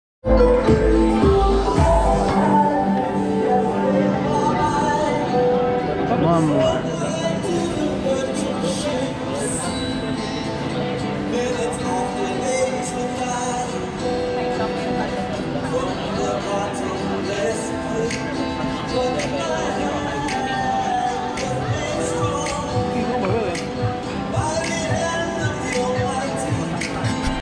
Sunset in Uluwatu - View from Single Fin
Just a quick shot with the GoPro Hero 7 Black as I walked out to the deck at Single Fin, Bintang in my other hand and Bob Marley in the air, to watch the sun come down in Bali on April 19, 2019.